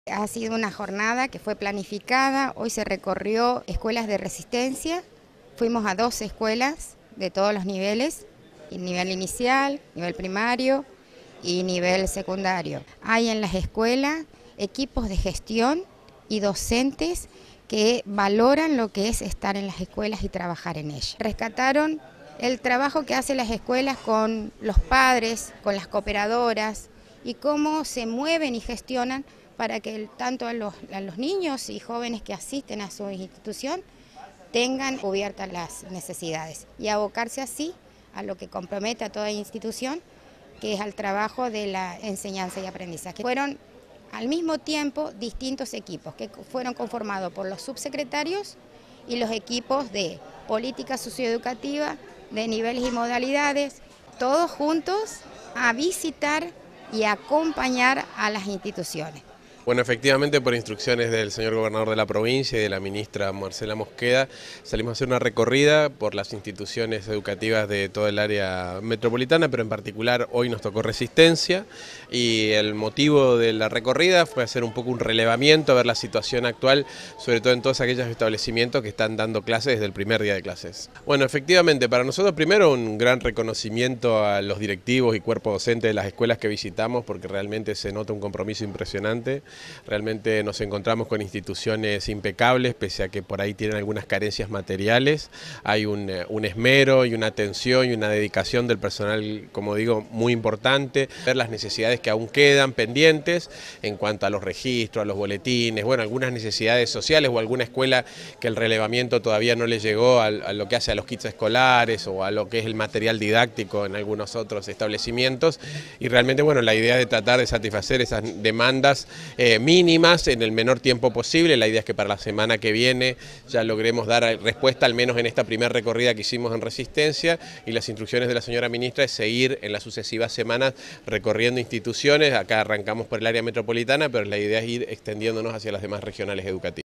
C9 - RESISTENCIA - Recorriendo Escuelas
INICIARON VISITAS A COMUNIDADES EDUCATIVAS DEL GRAN RESISTENCIA.
La ministra de Educación, Marcela Mosqueda, y los funcionarios de su gabinete esta semana se encuentran recorriendo distintas instituciones escolares de todos los niveles y modalidades del Gran Resistencia, con el fin de dialogar con las comunidades educativas y para acompañar a los docentes, en lo pedagógico, administrativo y comunitario.
 SONIA KRILICH SUBSECRETARIA DE CALIDAD Y EQUIDAD // EDGARDO PÉREZ SUBSECRETARIO DE RELACIÓN CON LA COMUNIDAD EDUCATIVA